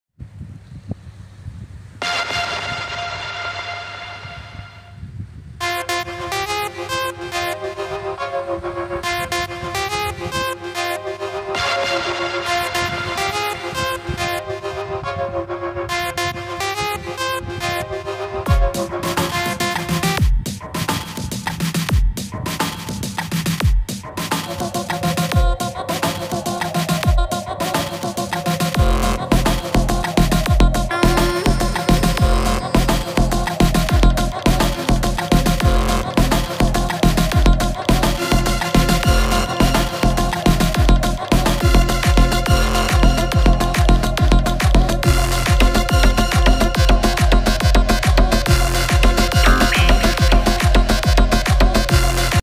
Does Conch (shell) work motivates us......
Chasing for a target, efforts from inside reflects and concentration of conch inspired me.